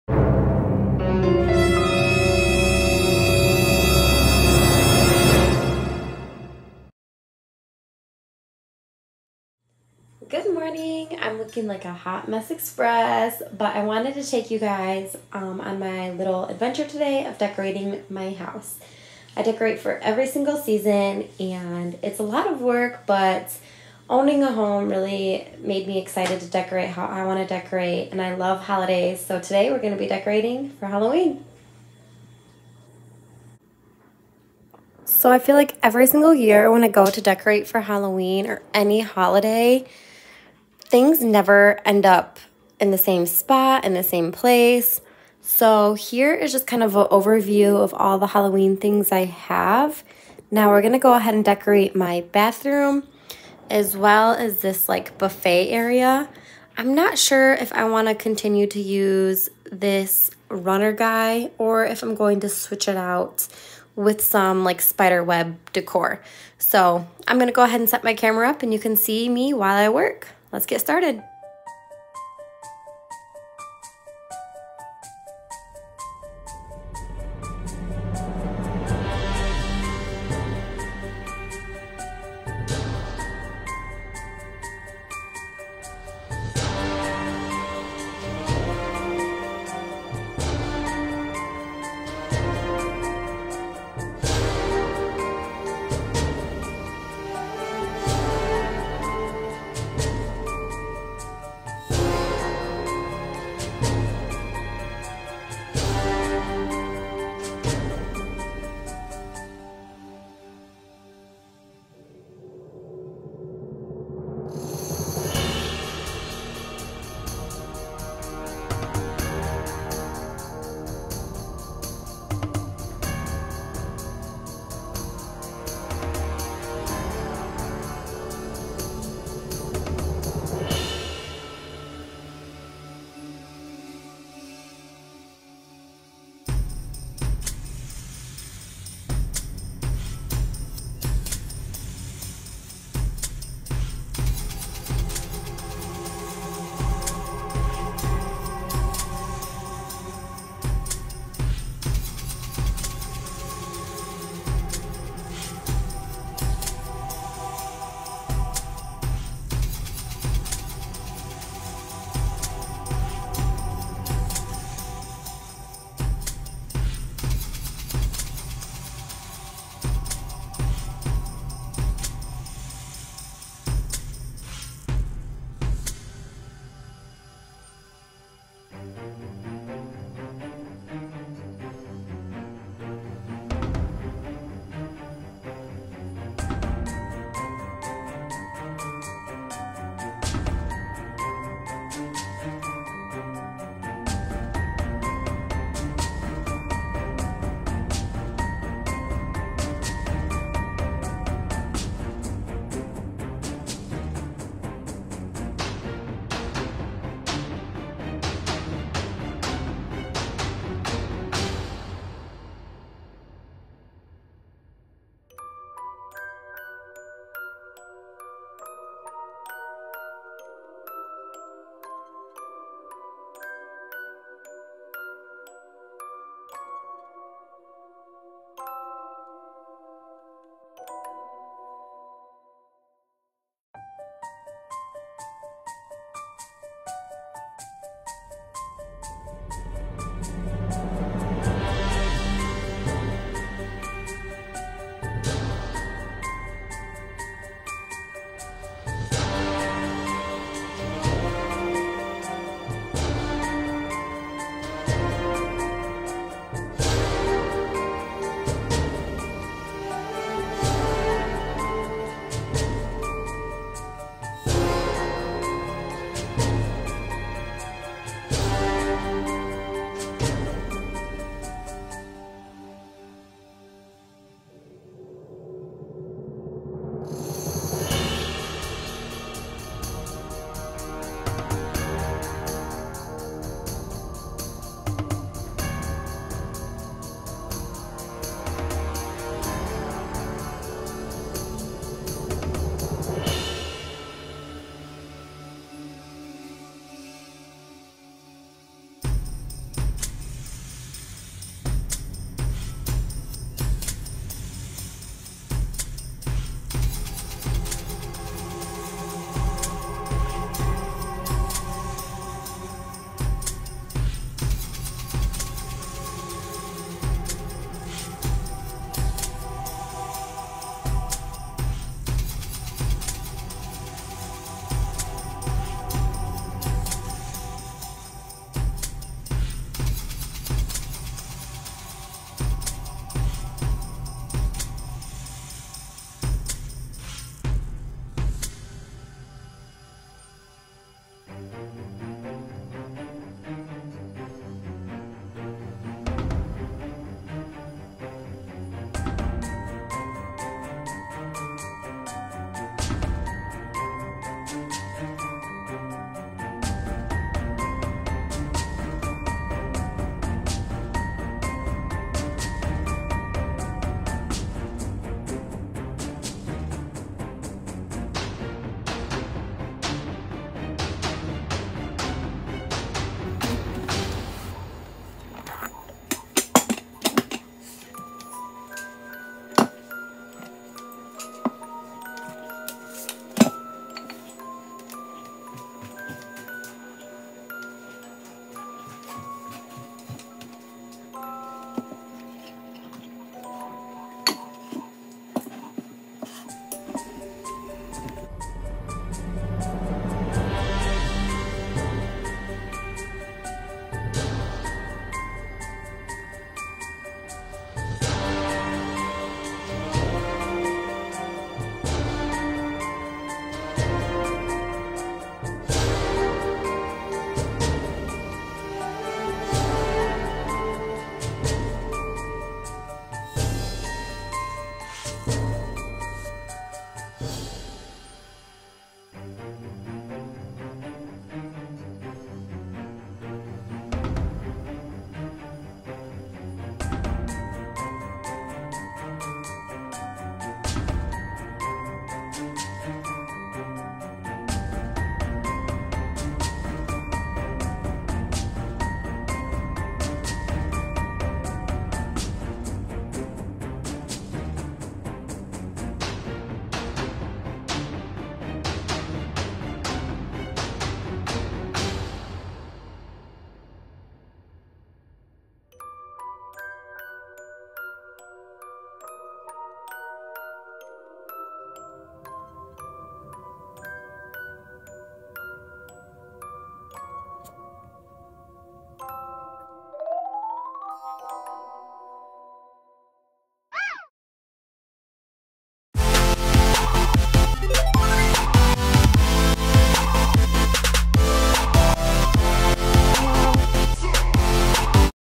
How to decorate for Halloween
Today I decided it was time to decorate for Halloween. I love decorating for every holiday! I hope you enjoy seeing how I decorate my house.

Use this link below to purchase some cute Halloween decor or anything else you would like,

Stay updated on my podcast, Life Unfiltered with Jo. You can go to my podcast playlist or search on Spotify, apple or google podcasts.